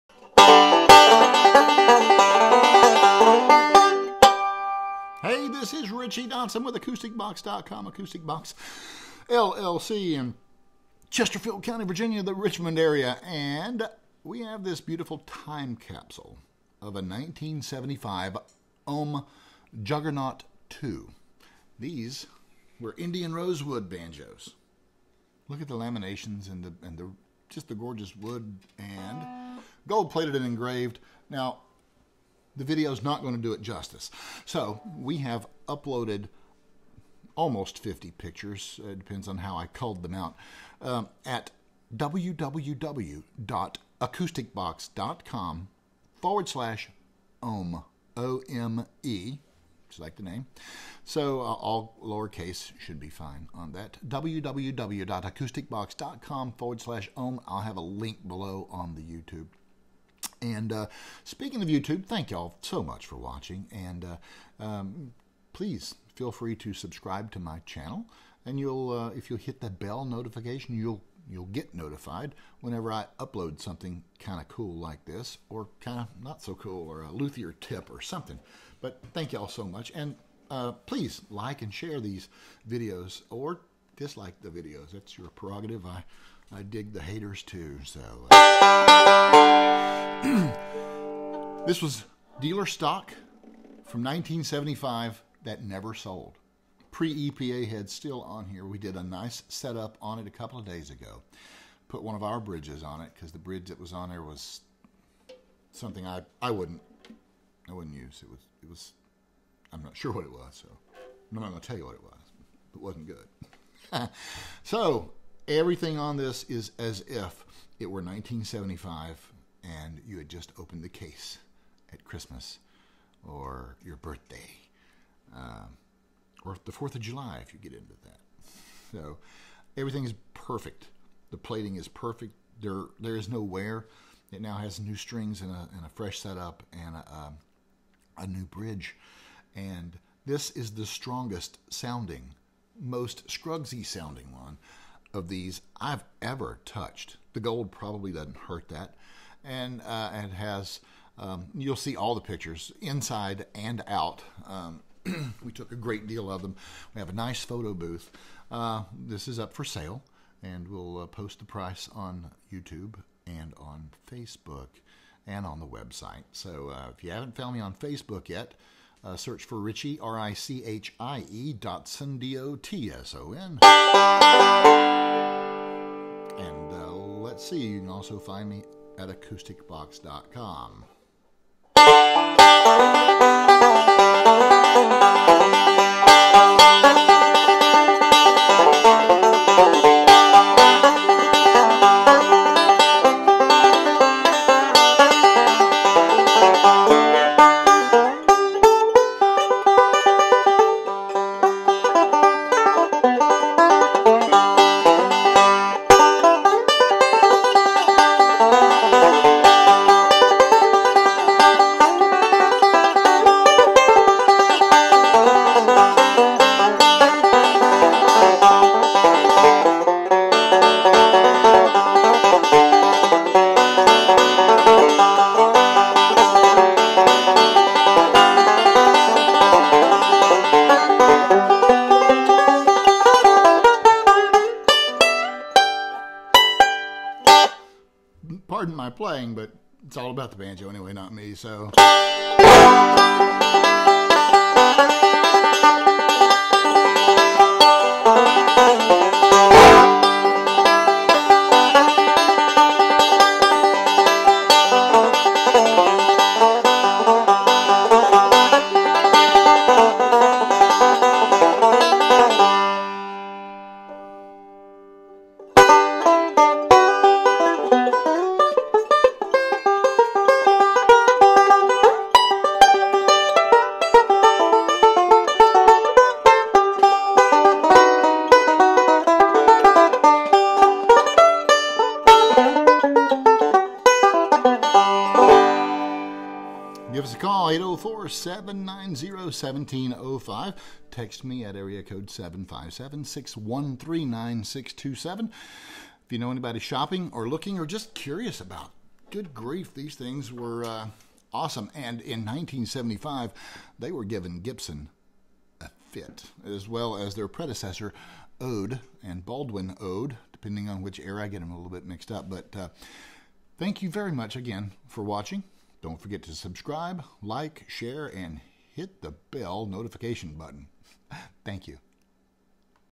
***SOLD*** 1975 Ome Juggernaut II For Sale New Old Stock ***SOLD***
***SOLD*** 1975 Ome Juggernaut II, Indian Rosewood 5-String Banjo in beautiful, heavy, 24 Karat gold plating and exquisitely engraved.
This banjo is an absolute time capsule! This was dealer stock that never sold. The 82 year old surviving member of the partnership brought this banjo to me 2 days ago and asked me to please set it up, photograph it and sell it for him. The warrantee card hasn't been filled out!

This beautiful 5-string banjo comes with the original hard shell case, the warrantee card and the original case key. It has a full compliment of spikes as you can see in the photographs and this banjo was never put into circulation. It is every bit as pristine as it looks!

I'll be posting these pictures on my website as well as doing a video soon. I did the setup yesterday and I have to say that this is the best sounding Ome I've ever had my hands on! WOW!!!